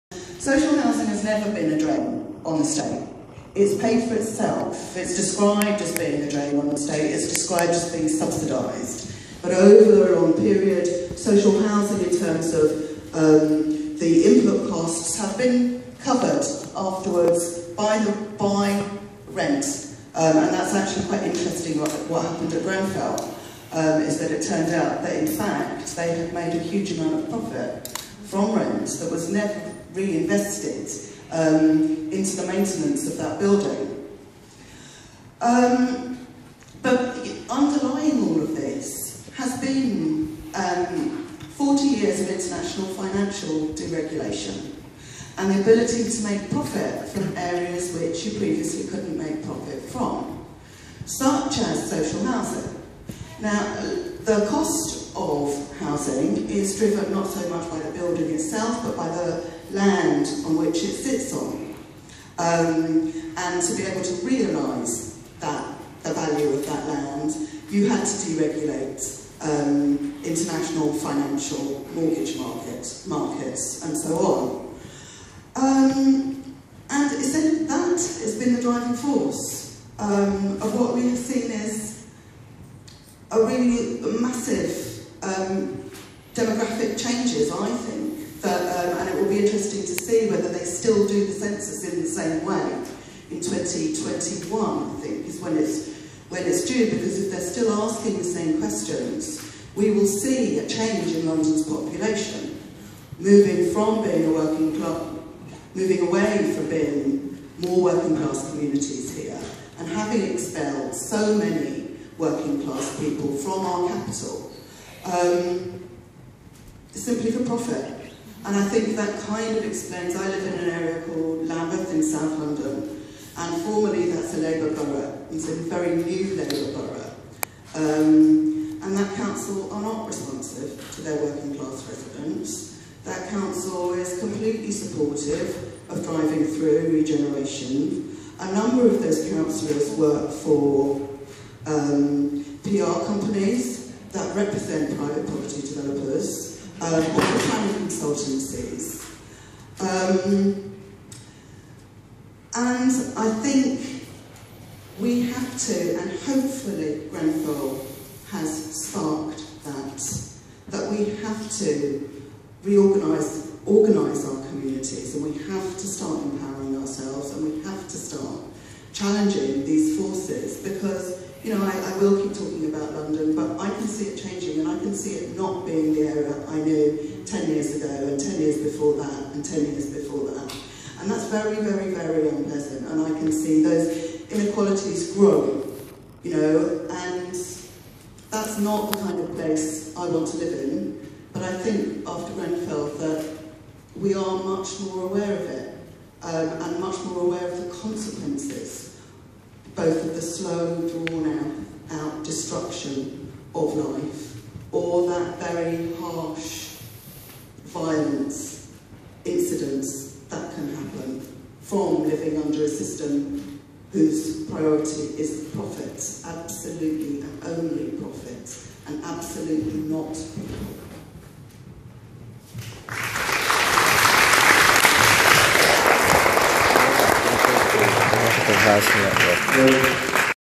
Radical Housing Network 2 - Liverpool Grenfell Conference at the Black-E
(filmed from the audience apologies for poor quality!)
Videos are not edited, just recorded as it was.
Thursday 3rd May 2018